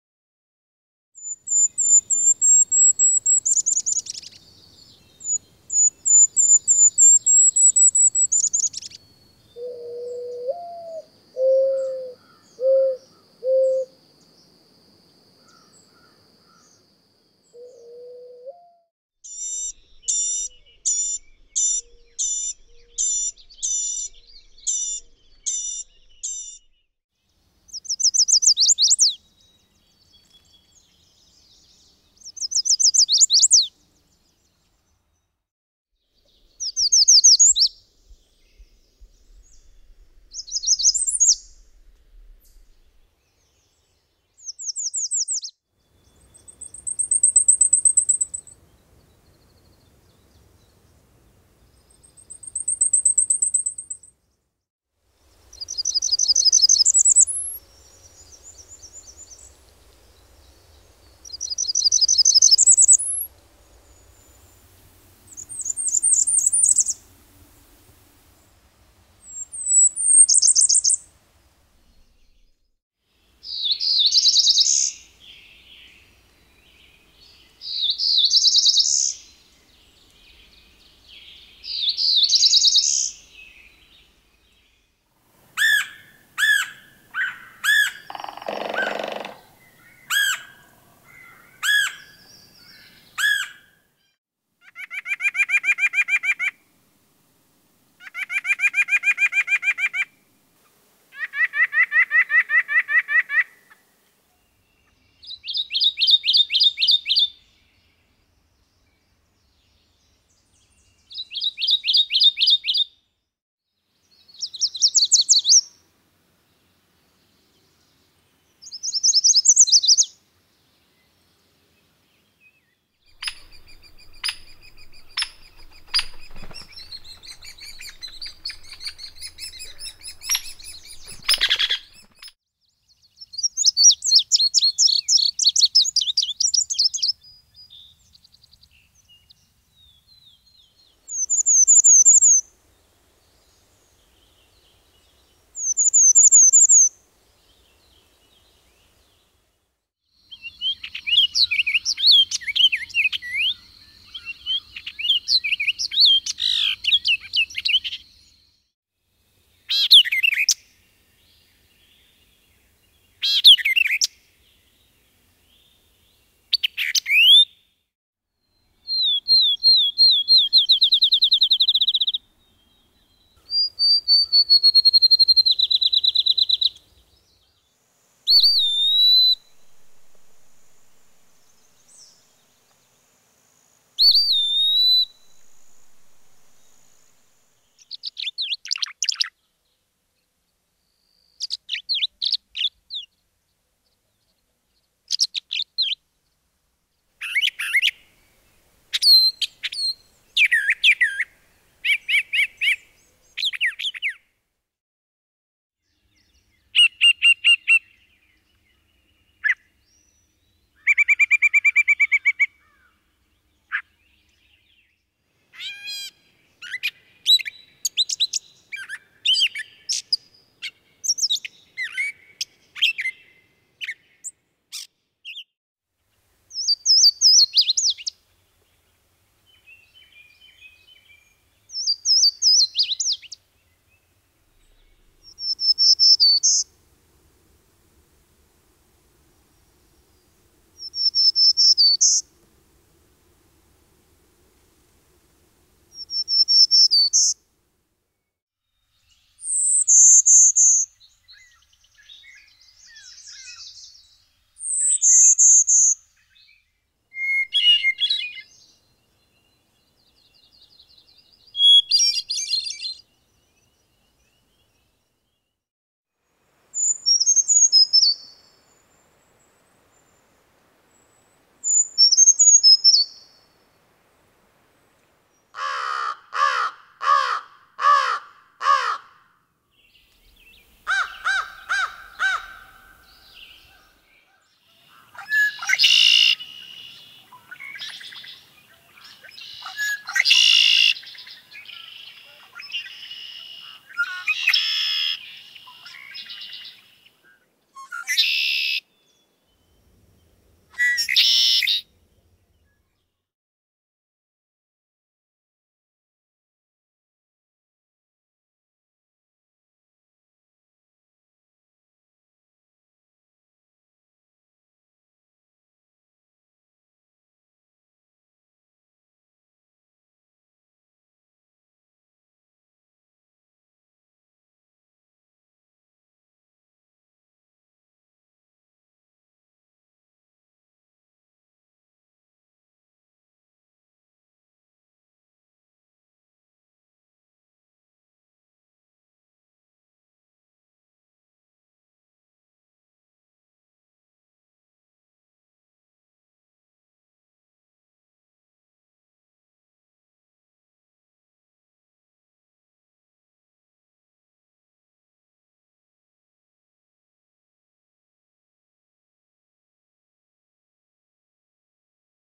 All About Birds
One of my favorite projects that I got to do with Radnor Lake State Park in Tennessee. This is a sneak peak of the film.